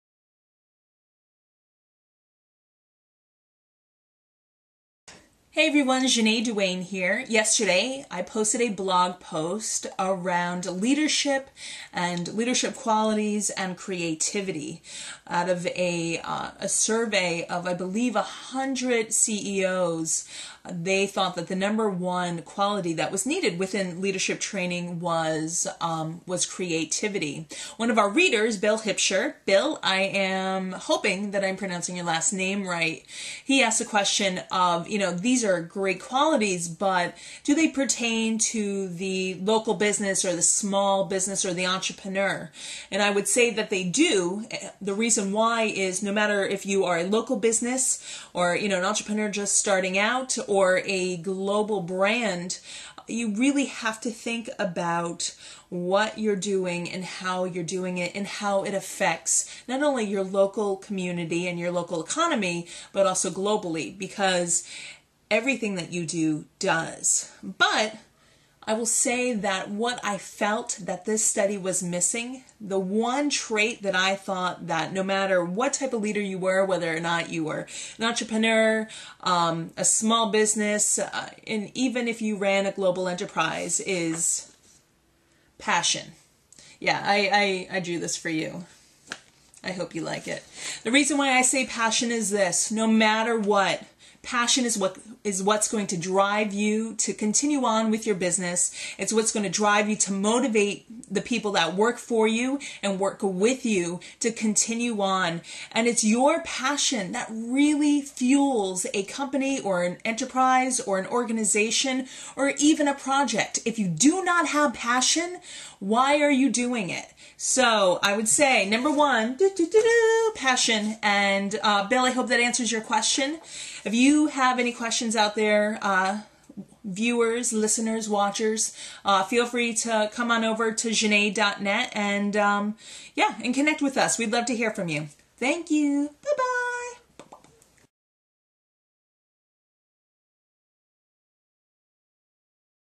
The Most Important Leadership Quality
A survey out of 100 CEOs stated that creative was the number one desirable trait in leadership. But, what about the entrepreneur or a leader running a project? Ja-Nae talks about the most important leadership quality that was missed in the survey.